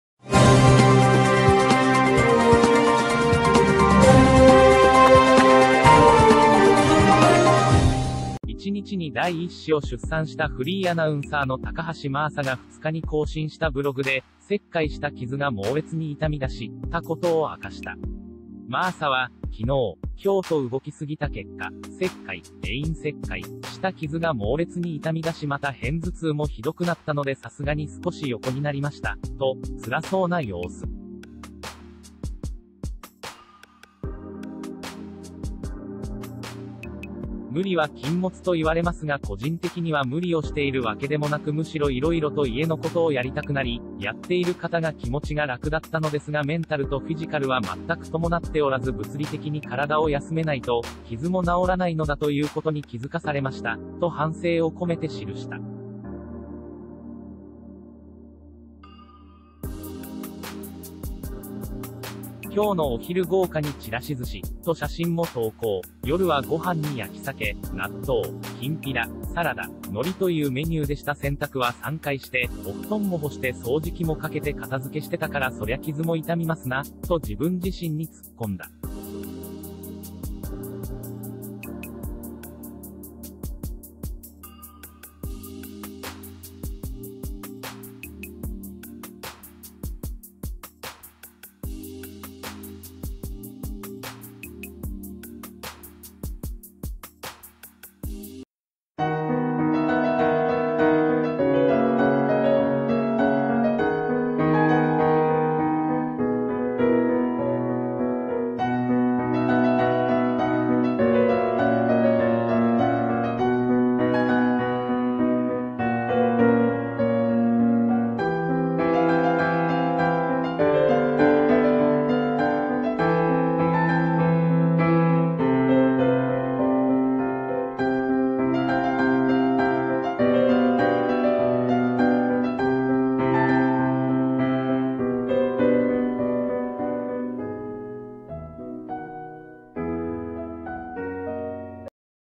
高橋真麻 切開した傷が猛烈に痛みだした…出産直後、動いたことを反省
１日に第１子を出産したフリーアナウンサーの高橋真麻が２日に更新したブログで、「切開した傷が猛烈に痛みだし」たことを明かした。  　真麻は「昨日、今日と動きすぎた結果、切開（会陰切開）した傷が猛烈に痛みだし　また偏頭痛もひどくなったので　さすがに少し横になりました」と、つらそうな様子。  　「無理は禁物　と言われますが個人的には　無理をしているわけでもなく　むしろ色々と家の事をやりたくなり、やっている方が気持ちが楽だったのですが　メンタルとフィジカルは全く伴っておらず　物理的に身体を休めないと、傷も治らないのだということに気付かされました」と反省を込めて記した。  　「今日のお昼　豪華にちらし鮨」と写真も投稿。「夜は　ご飯に焼き鮭、納豆、金平、サラダ、海苔というメニューでした　洗濯は３回して、お布団も干して　掃除機もかけて　片付けしてたから　そりゃ傷も痛みますな」と自分自身にツッコんだ。